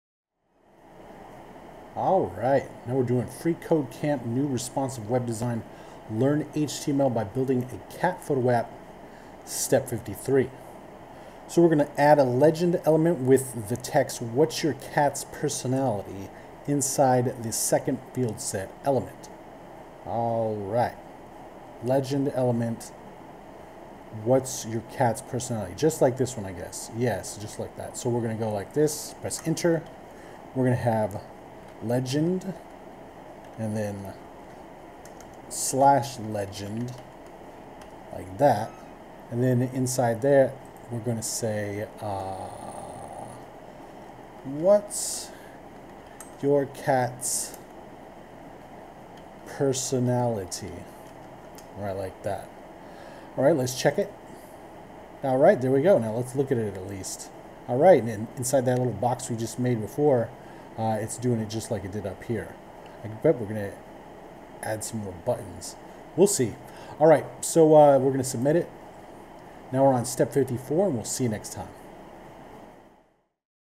Learn HTML by Building a Cat Photo App - Step 53
this one has us adding another legend element